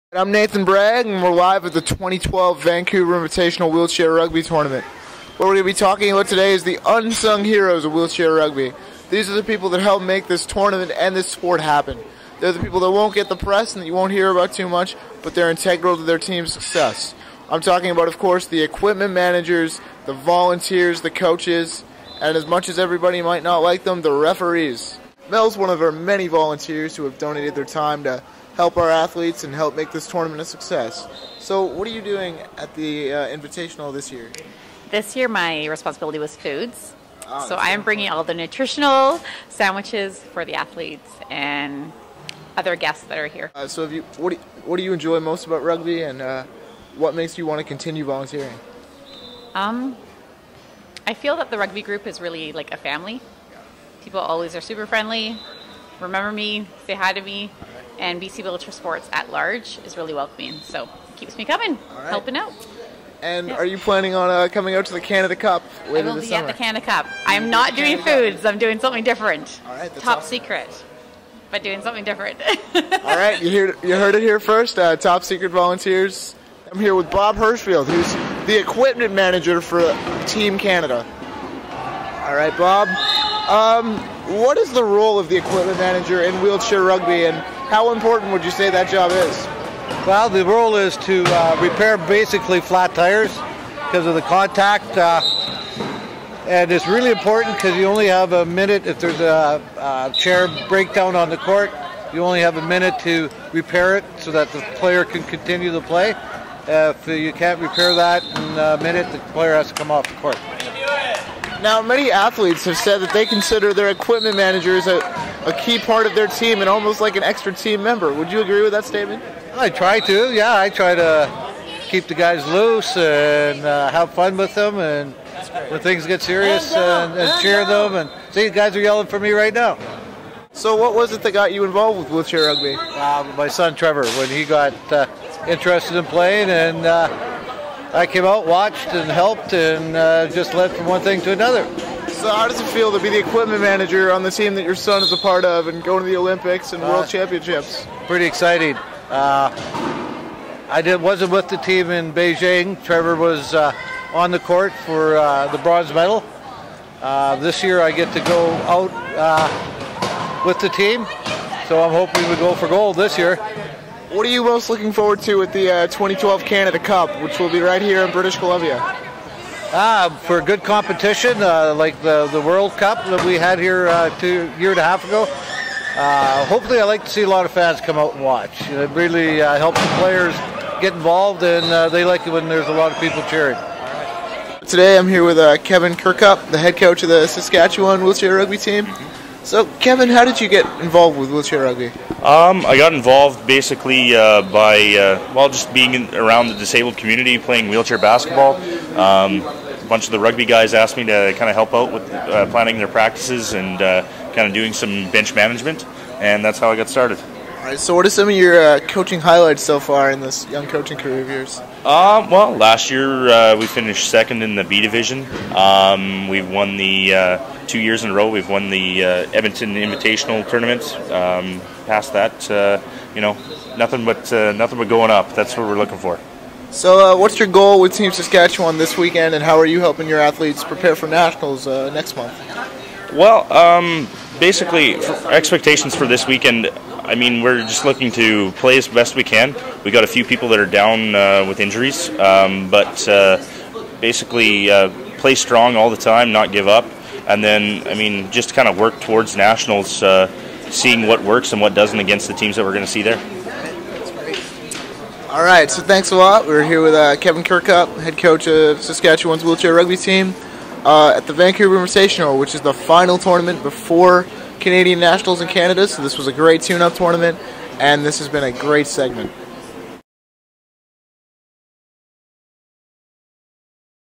The Unsung Heroes of Wheelchair Rugby
In the second instalment of the Vancouver Invitational Wheelchair Rugby Video Blog, BC Wheelchair Rugby Reporter Nathan Bragg goes behind the scenes to celebrate the unsung heroes of wheelchair rugby.